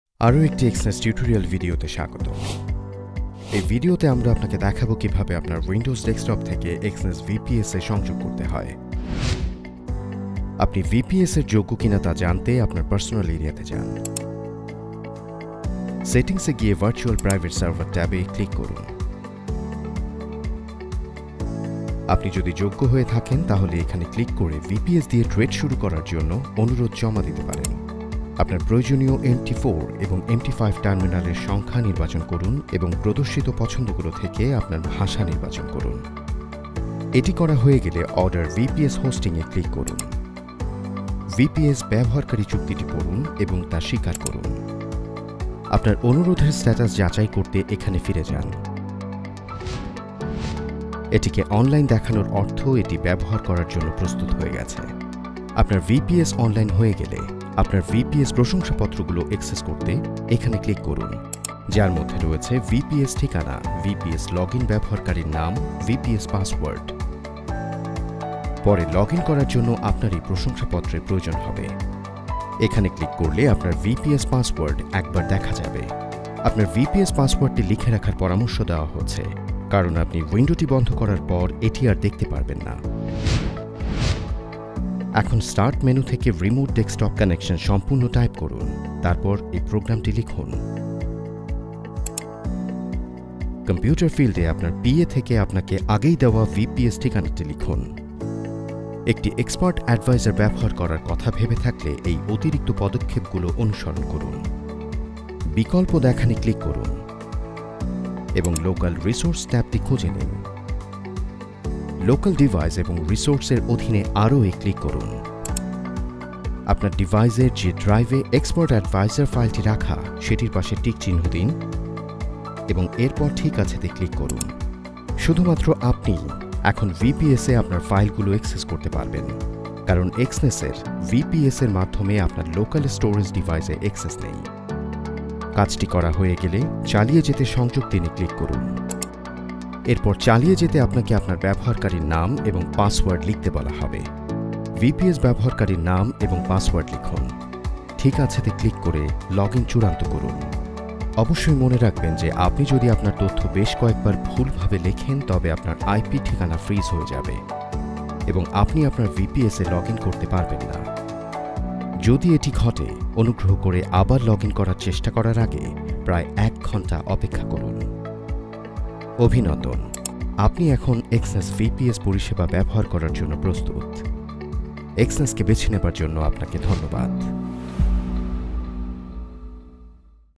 কিভাবে Windows-এর জন্য Exness VPS সংযোগ করবেন
Exness সেই সকল ট্রেডারদের অফার করে যারা আমাদের ফ্রি ভার্চুয়াল প্রাইভেট সার্ভারে (VPS) অ্যাক্সেসের যোগ্যতা অর্জন করে। আজ কিভাবে উইন্ডোজের জন্য VPS ব্যবহার শুরু করবেন তা দেখুন।